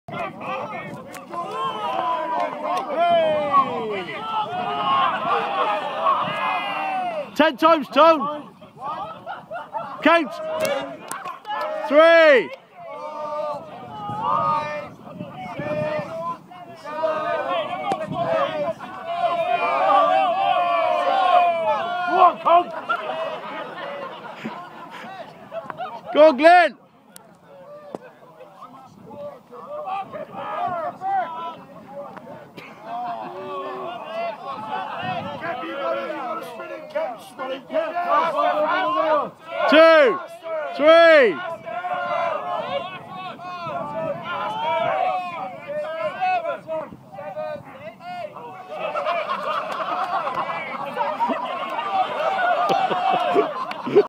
Spartans Rugby Tour Blackpool April 2012
This is a video of some of my friends in Blackpool on rugby tour..!! A must watch lol